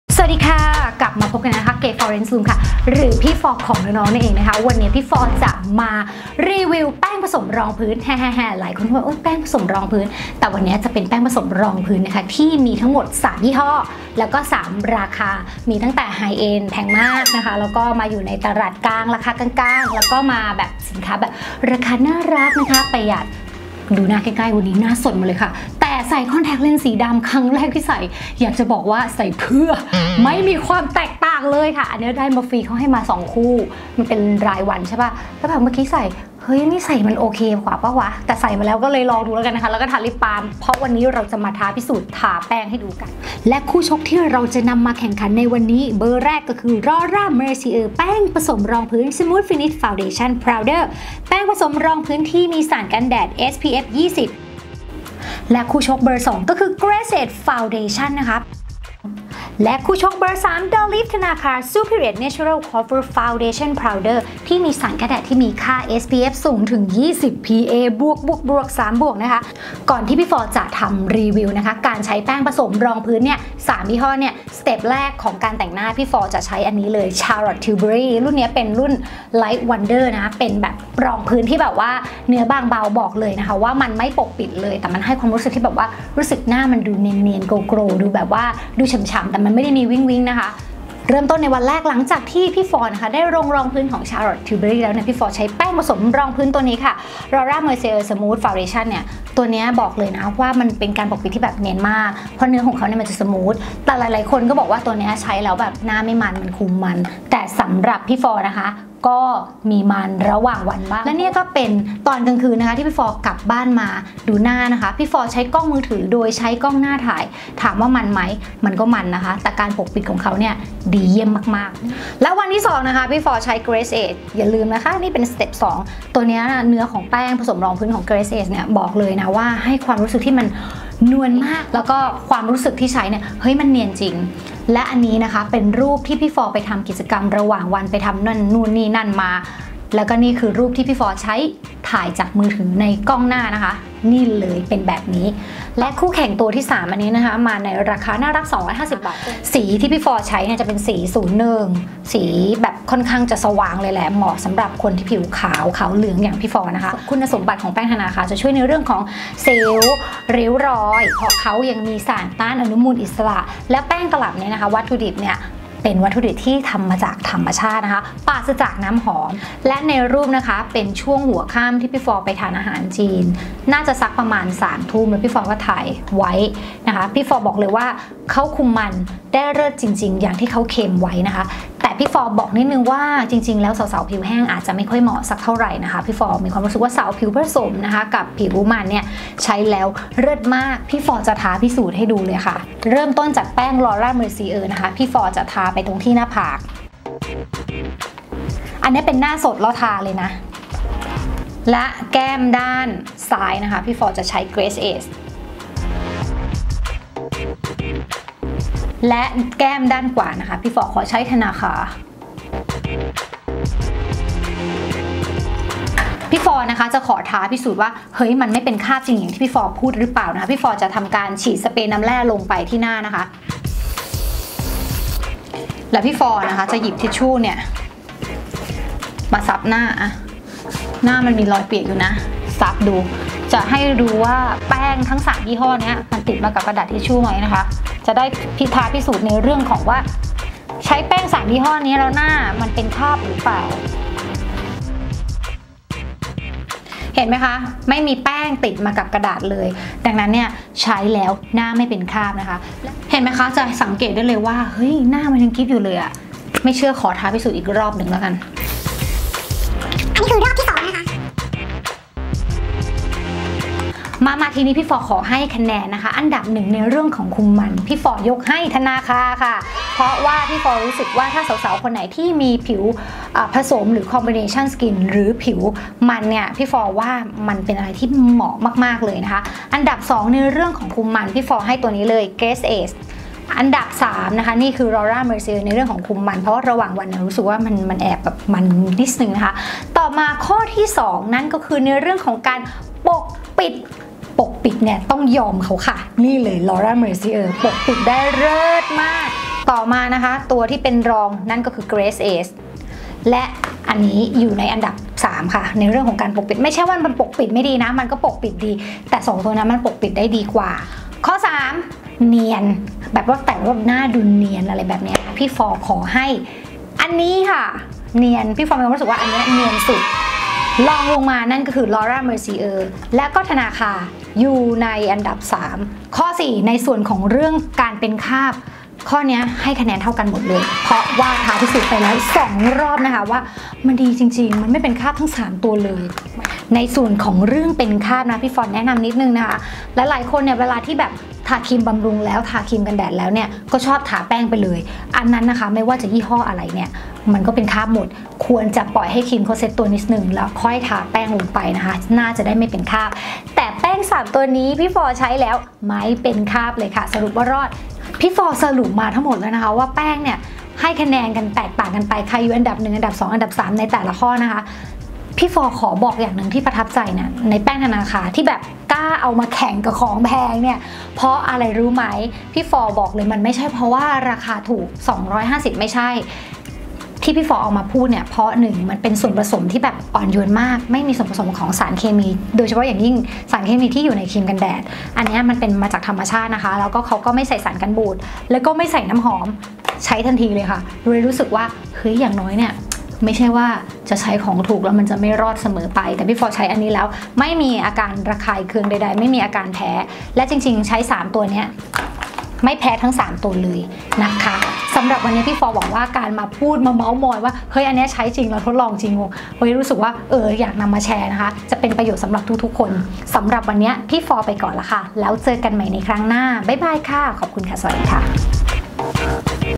รีวิว : แป้งผสมรองพื้นเปรียบเทียบราคา แพง กลาง ถูก อันไหนเริ่ดต้องดู I Florence room beauty
สวัสดีคะคลิปนี้เป็นคลิปเจาะลึกรายละเอียดแป้งผสมรองพื้น โดยมีทั้งหมด 3 ราคา มีตั้งแตาราคาแพง ราคากลางๆ และราคาถูก แต่ละแบรนด์ก็มีจุดเด่นที่แตกต่างกันไป และ นี่เป็นความรู้สึกของพี่ฟลอเพียงคนเดียวเท่านั้น อยากรู้อันไหนเริ่ดต้องดูคร้า สามารถติดตามพี่ฟลอได้ที่